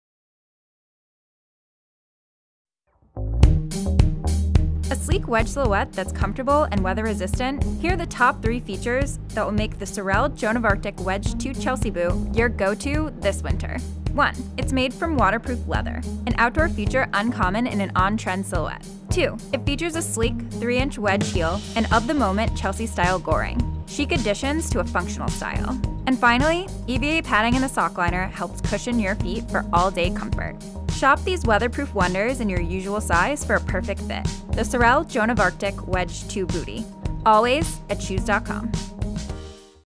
Sorel Joan of Arctic Wedge II Chelsea | Shoes.com
A wedge silhouette that’s comfortable and weather-resistant. Here are the top three features that will make the Sorel Joan of Arctic Wedge II Chelsea boot your go-to this winter.

One, it’s made from waterproof leather, an outdoor feature uncommon in an on-trend silhouette.

Two, it features a sleek 3-inch wedge heel and of-the-moment Chelsea-style goring. Stylish additions to a functional style.
And finally, EVA padding in the sock liner helps cushion your feet for all-day comfort.

Shop these weather-proof wonders in your usual size for a perfect fit. The Sorel Joan of Artic Wedge II, always at Shoes.com.

Check out the Sorel Joan of Arctic Wedge II Chelsea